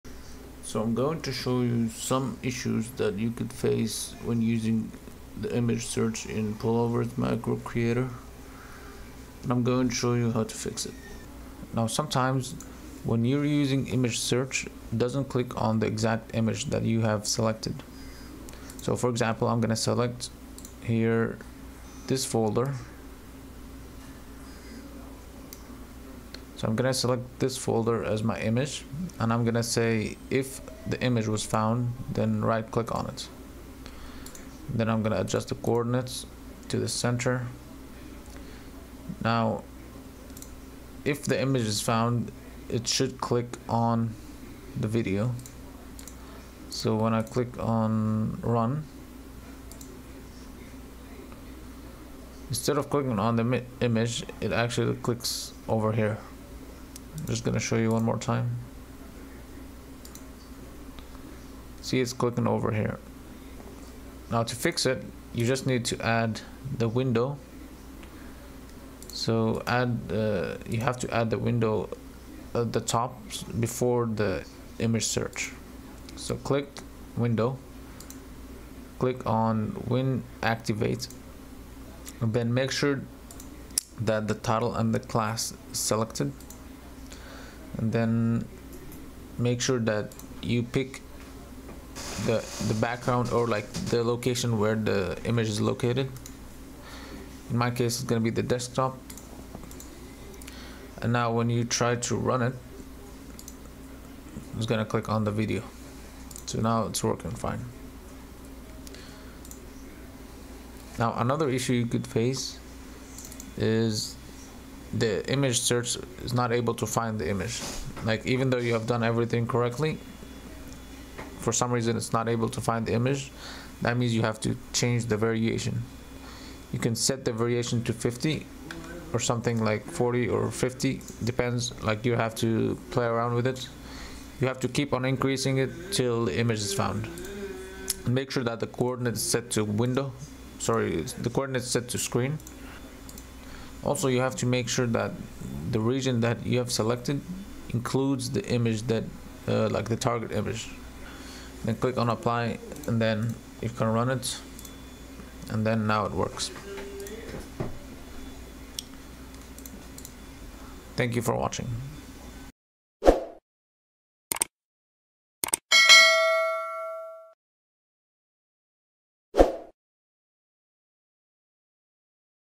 Pulover's Macro creator (Image Search Not Working - FIXED)
Having issues with Image Search not working in Pulover’s Macro Creator? In this video, I show you exactly how to fix it! Whether your image isn’t being detected or nothing happens when you run the macro, I’ll walk you through the steps to get it working again — fast and simple.